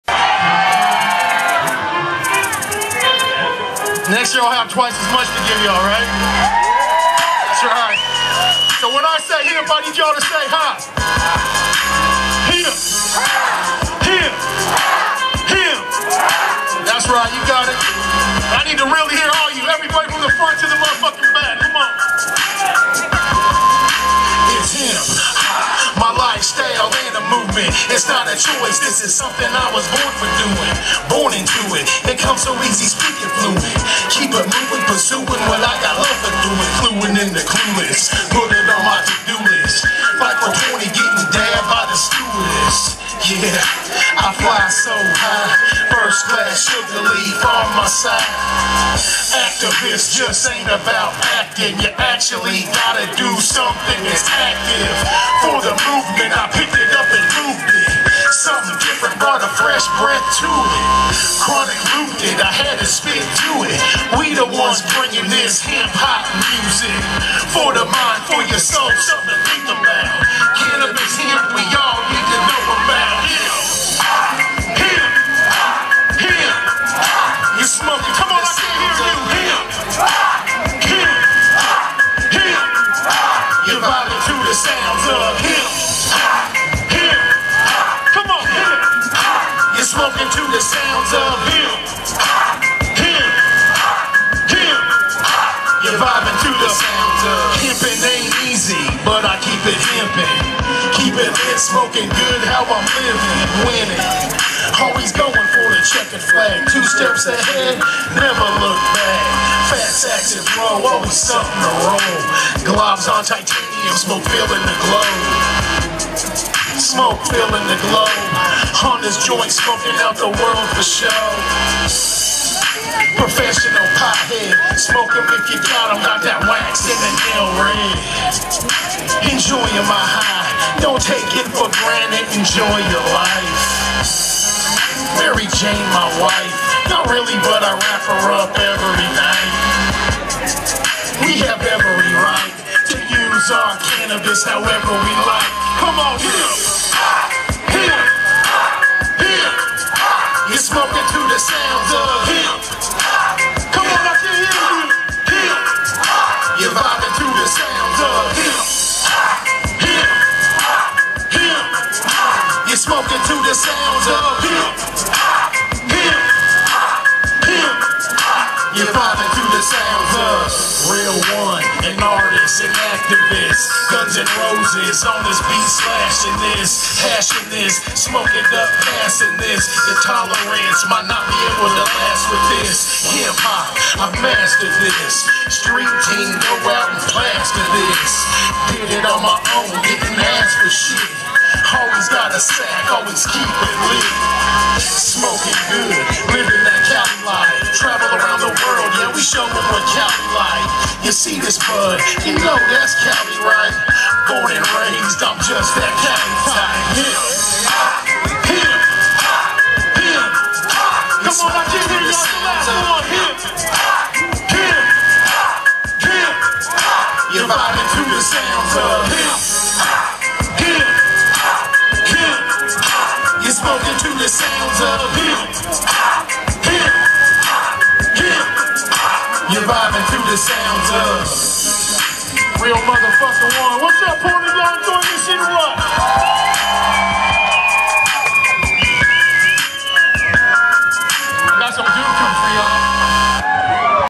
Real One - Track 2 - Live from Hempstalk 2014
This is Real One performing on the Hempstalk main stage on Sunday September 28th 2014 at Tom McCall Waterfront Park in Portland Oregon.

Thanks to all the volunteers and organizers of this event. We all know it was extra hard to pull this event together this year.